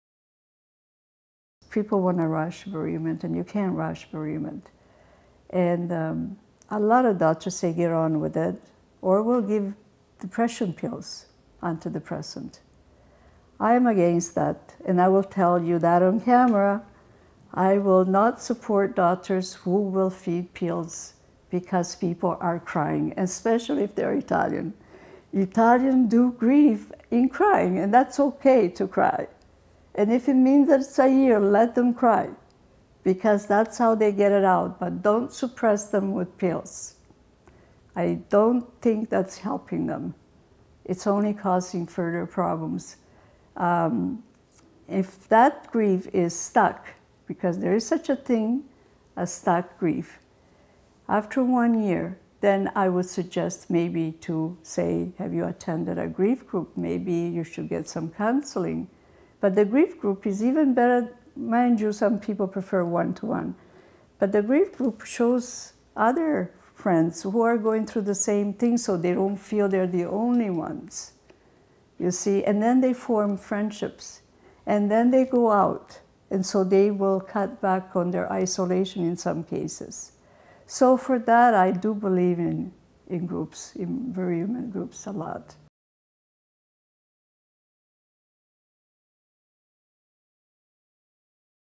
Francesca: Grief can't be rushed
In this video by Canadian Virtual Hospice, Francesca discusses grief and sadness after a death as normal.